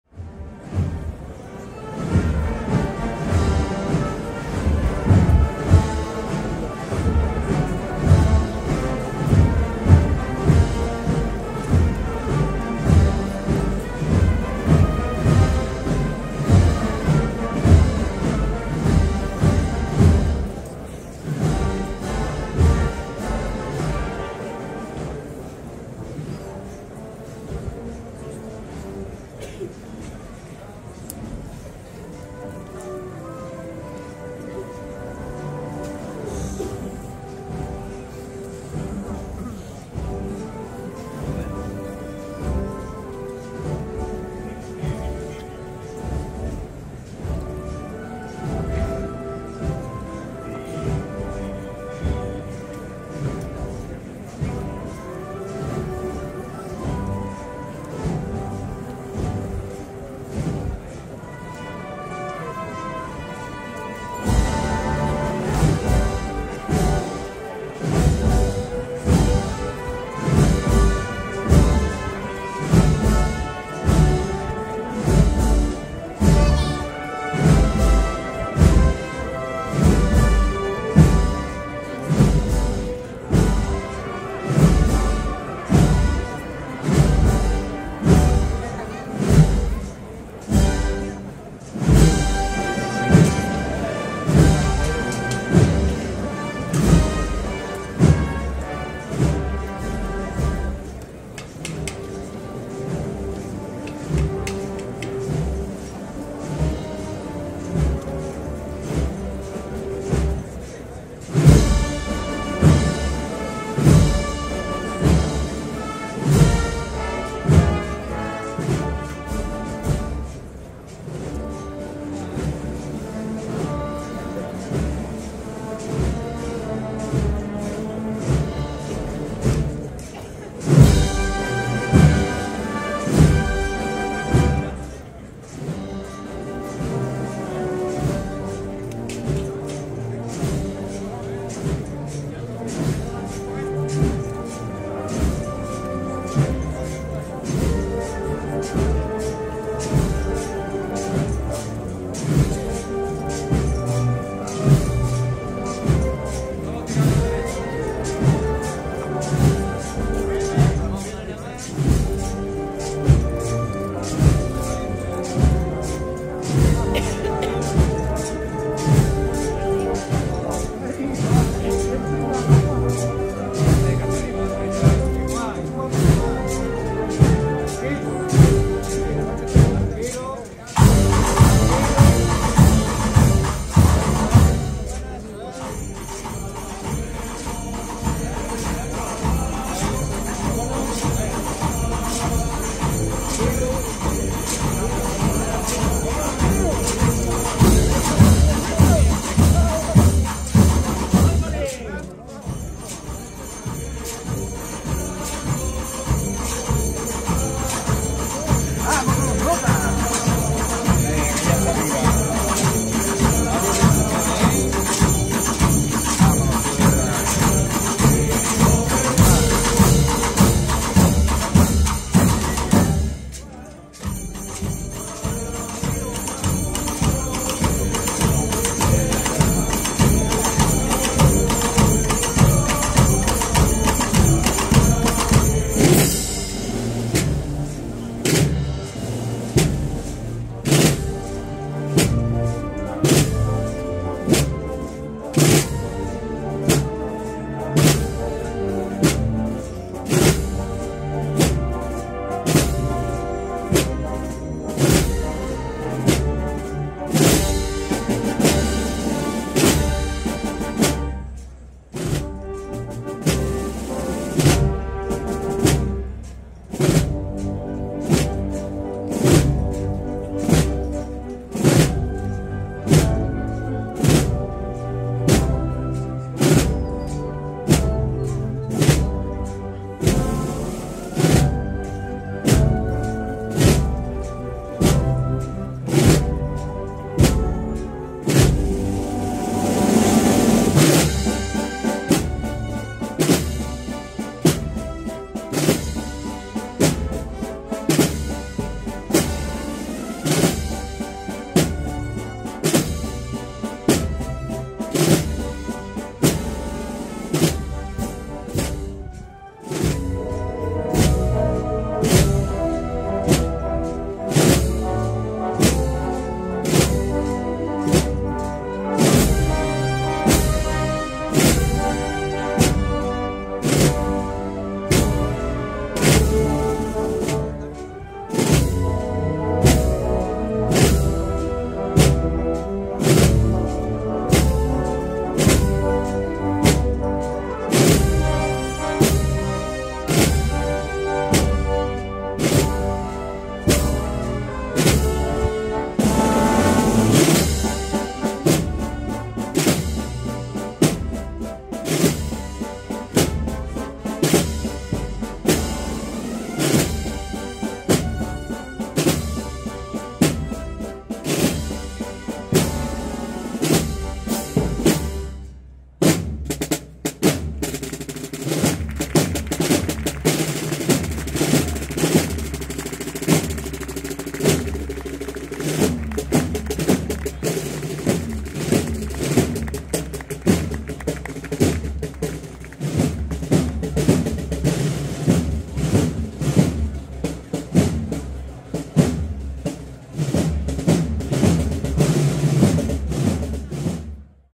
Dulce Nombre por la Cuesta del Bacalao | Sevilla 2024
✝️​ María Santísima del Dulce Nombre
🎺 Sociedad Filarmónica de la Oliva de Salteras (Sevilla)
📍​  Cuesta del Bacalao

Toda publicación que se haga de este contenido deberá mencionarse "Pasión de Cofrades"